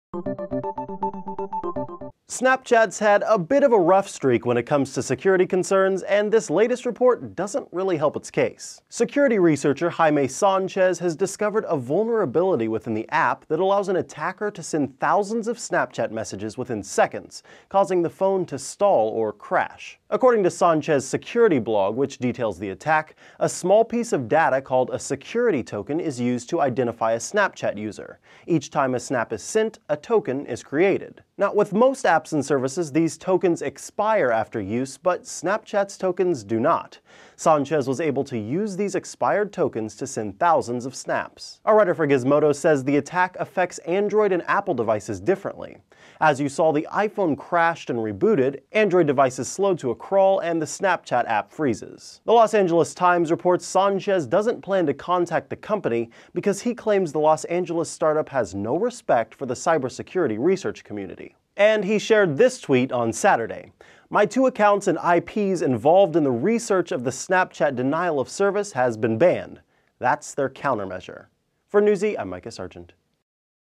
Hackers Can Use Snapchat To Disable, Crash Phone
A security researcher has discovered a flaw within Snapchat's app that could allow an attacker to disable or crash a phone.